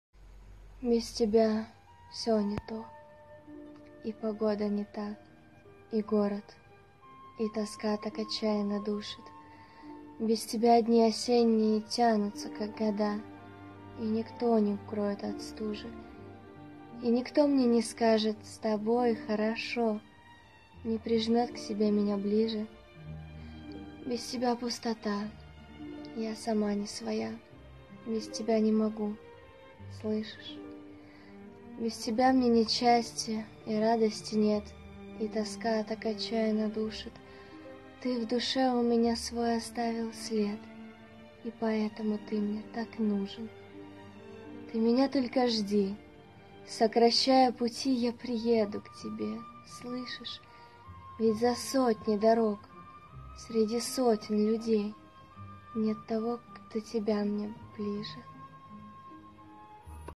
Без тебя все не то... | Автор Наталья Малёнкина
Читает: Наталья Малёнкина (Instagram: @natalimurmur11)

Без тебя все не то: и погода не та, и город, и тоска так отчаянно душит.
Без тебя дни осенние тянутся, как года, и никто не укроет от стужи.
И никто мне не скажет: «С тобой хорошо», не прижмет к себе меня ближе.
Без тебя пустота , я сама не своя, без тебя не могу, слышишь?
Без тебя мне нет счастья и радости нет, и тоска так отчаянно душит,
Ты в душе у меня свой оставил след, и поэтому ты мне так нужен…
Ты меня только жди, сокращая пути, я приеду к тебе, слышишь?
Ведь за сотни дорог, среди сотен людей нет того, кто тебя мне ближе...
Наталья Малёнкина